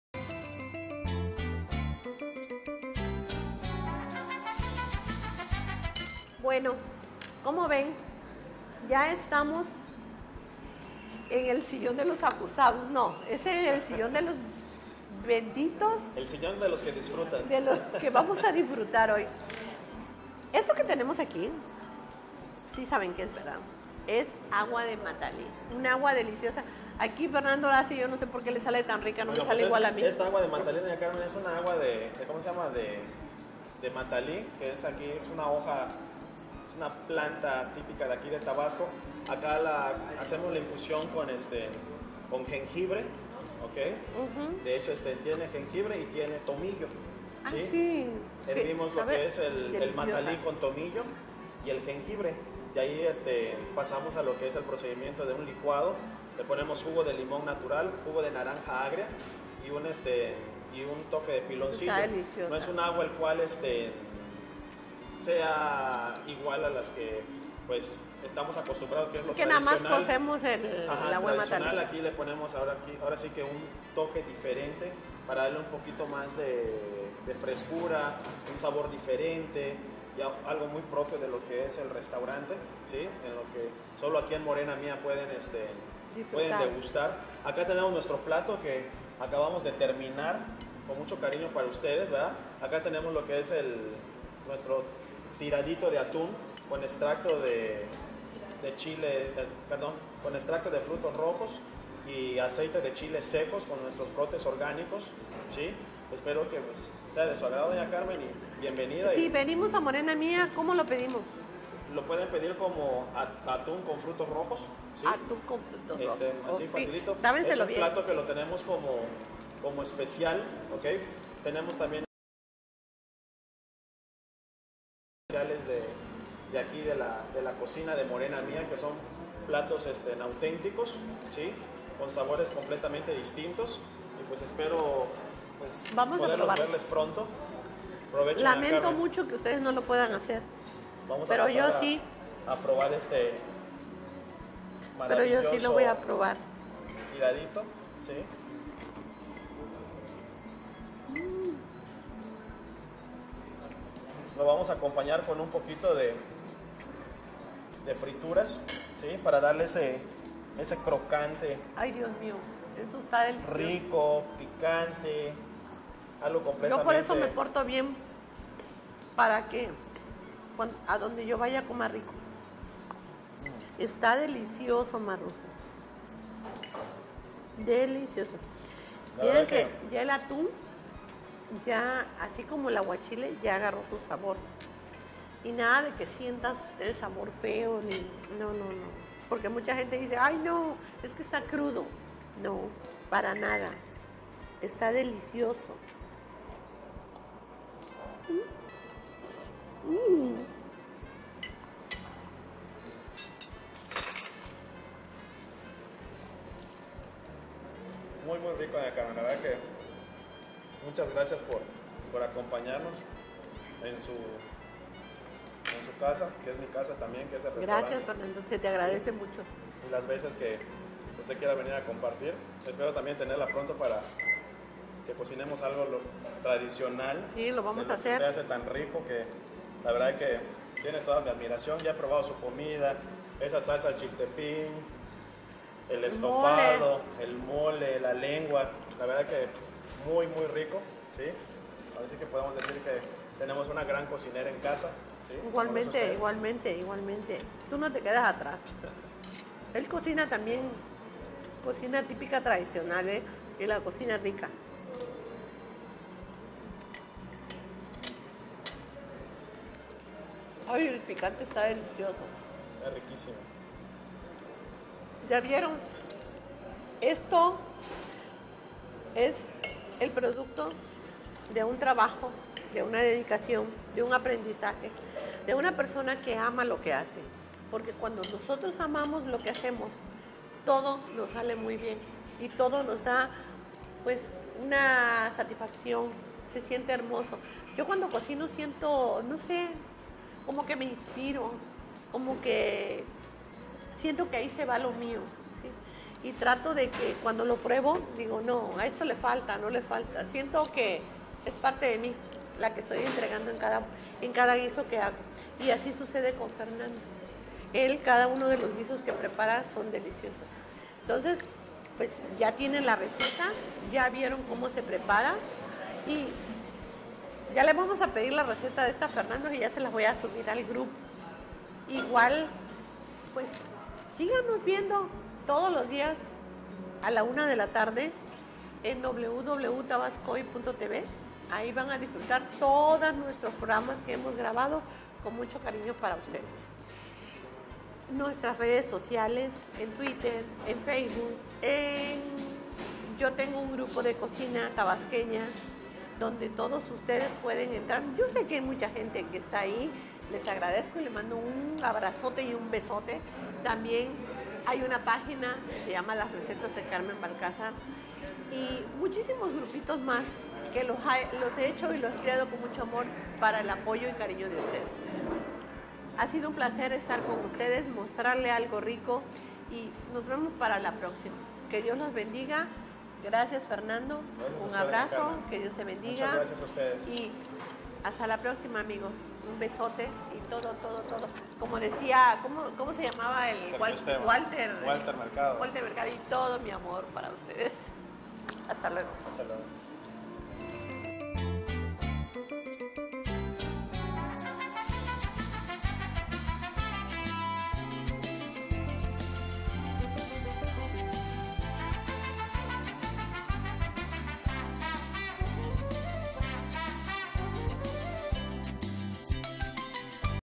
Cocina Tabasqueña Lomo de Atún con frutos Rojos Parte 41
EN MORENA MIA, DONDE TODO ESTA DELICIOSO, UNA RICA ENTREVISTA CON UN GRAN CHEF, FERNANDO MARRUJO,